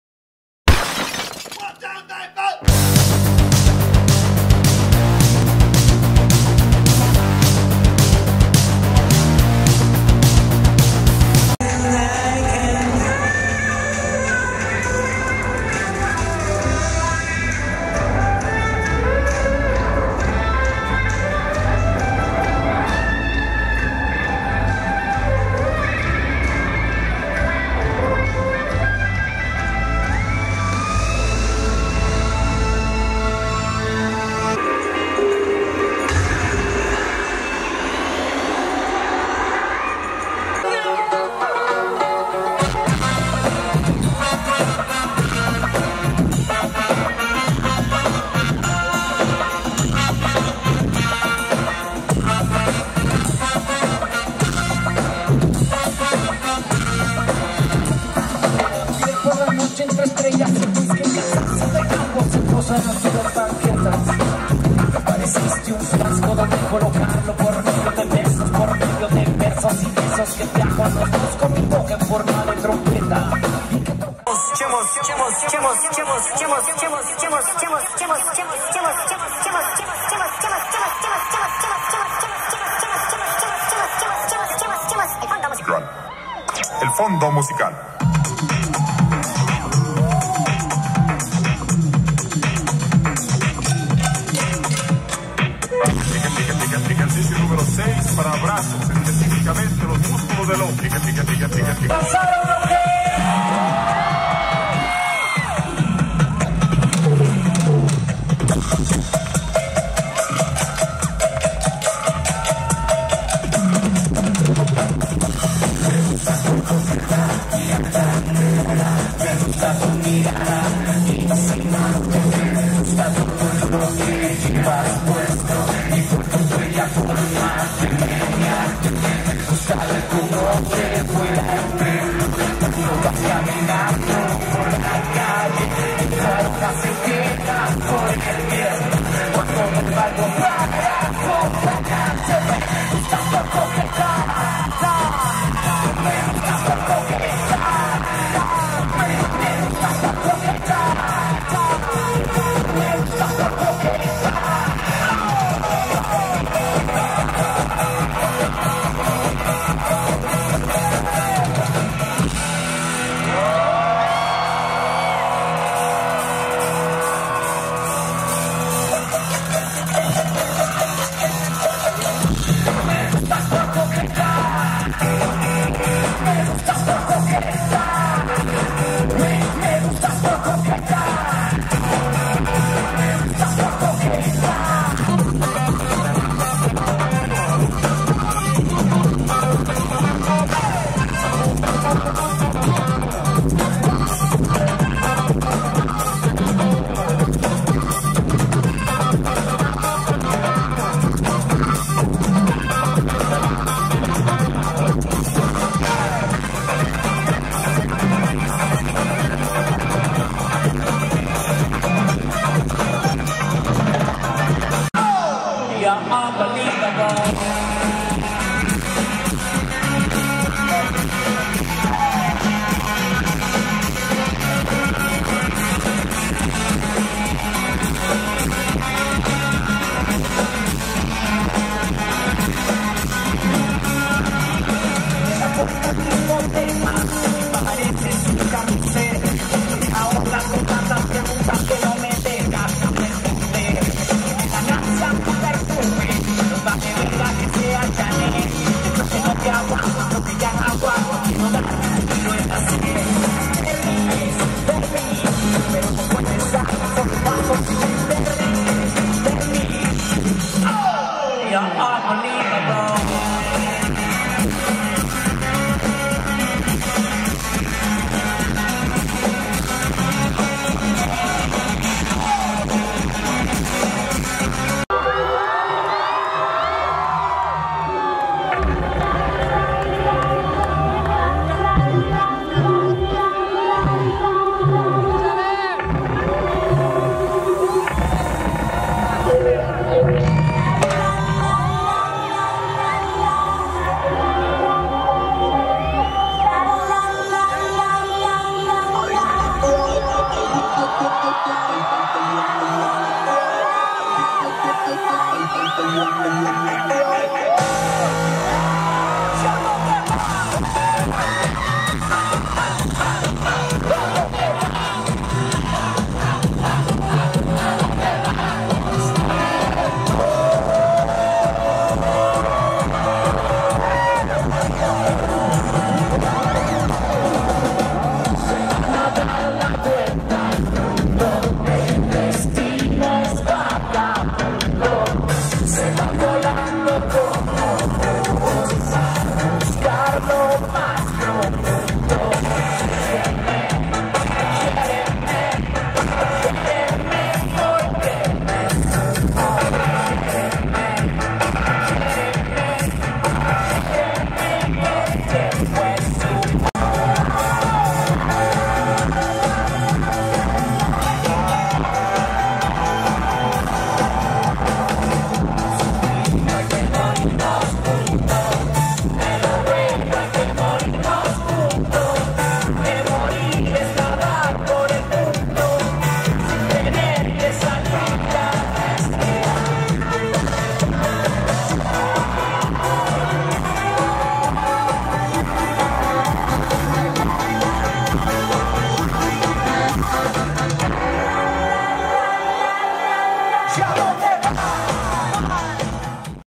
KINKY MAPPING MORELIA (FIMA) Festival Internacional de Mapping
KINKY en la Clausura del Festival Internacional del Mapping en Morelia Michoacan Plaza Valladolid.